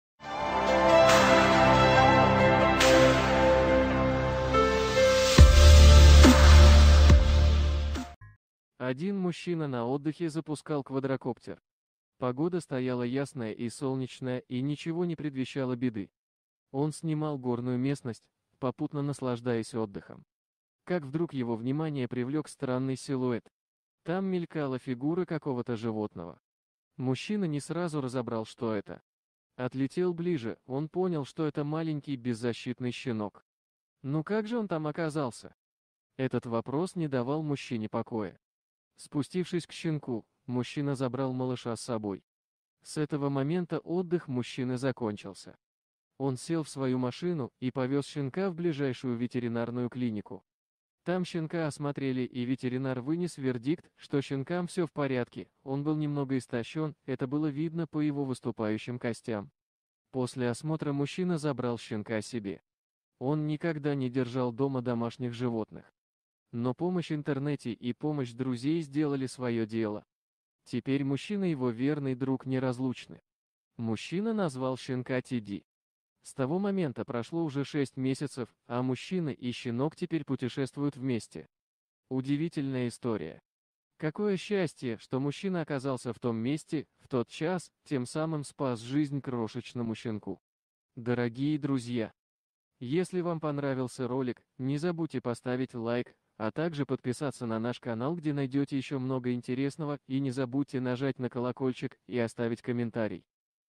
Беспилотник заснял то, что потрясло весь мир. Никто не должен был этого видеть!
Один мужчина на отдыхе запускал квадрокоптер. Погода стояла ясная и солнечная и ничего не предвещало беды. Он снимал горную местность, попутно наслаждаясь отдыхом. Как вдруг его внимание привлёк странный силуэт. Там мелькала фигура какого-то животного. Мужчина не сразу разобрал что это.